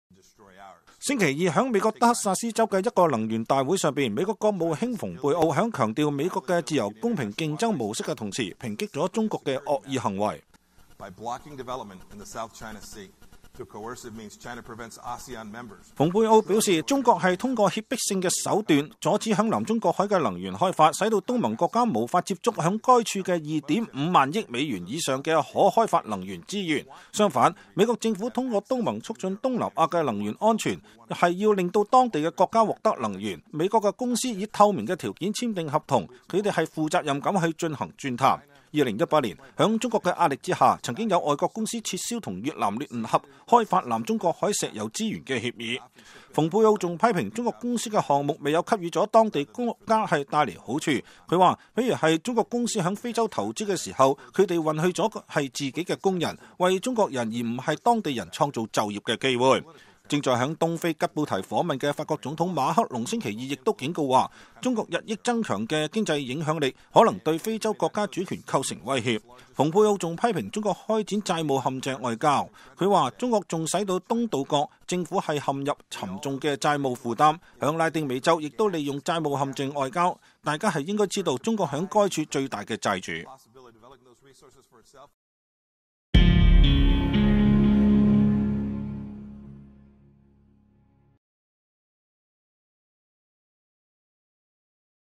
蓬佩奧稱中國壟斷南中國海能源並實行債務陷阱外交
星期二在美國德克薩斯州的一個能源大會上，美國國務卿蓬佩奧在強調美國的自由公平競爭模式的同時，抨擊了中國的惡意行為。他說，“中國通過脅迫性的手段阻止在南中國海的能源開發，使東盟國家無法接觸那裡的2.5萬億美元以上的可開發能源資源。相反，美國政府通過東盟促進東南亞的能源安全。我們要讓當地的國家獲得能源。我國公司以透明的條件簽訂合同，我們負責任地進行鑽探。”